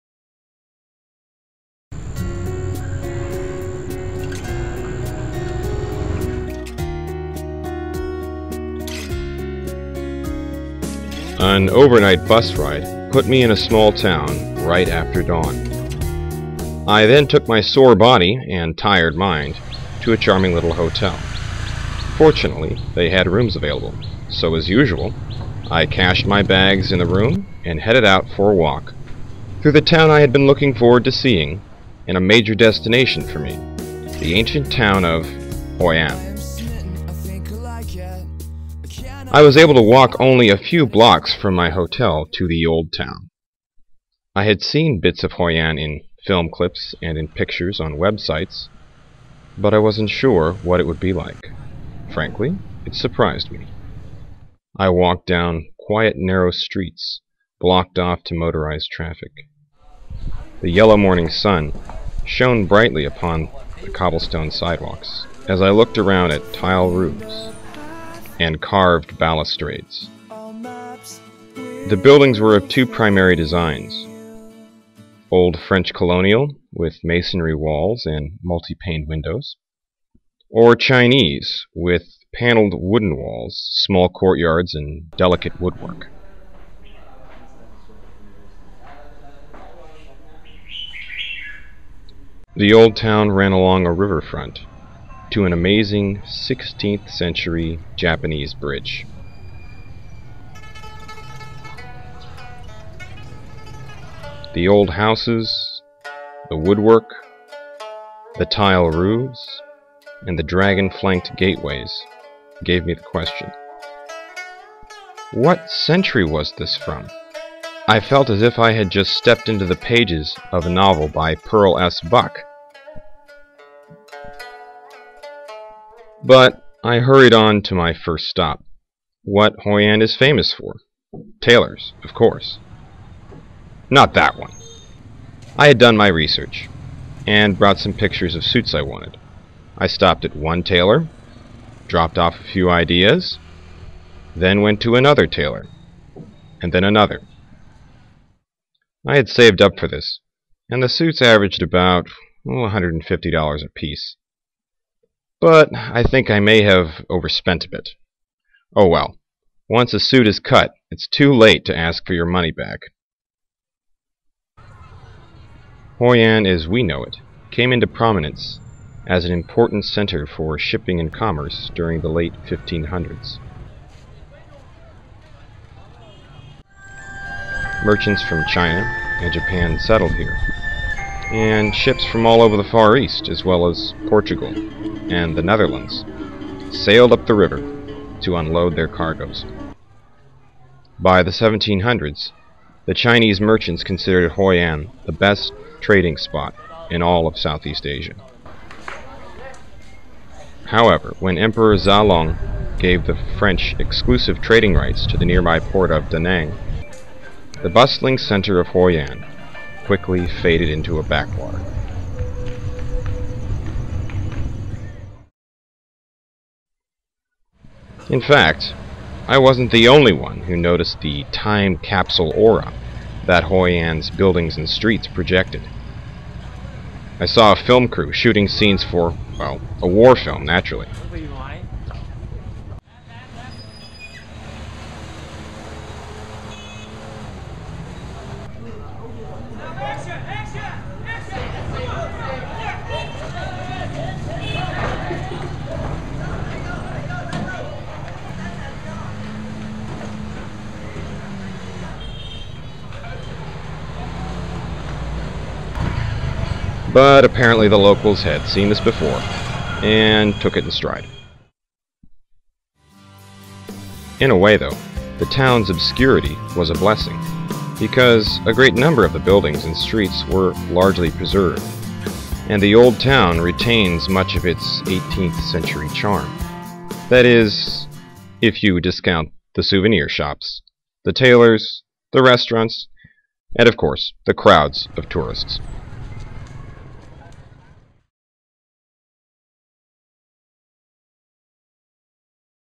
Impress Hoi An Vietnam
Hoi An Vietnam, a world heritage site recognized by UNESCO is gradually capturing the heart of domestic and international tourists by its peaceful beauty, unique architectures, special cultural cuisine, beautiful beaches and kind people.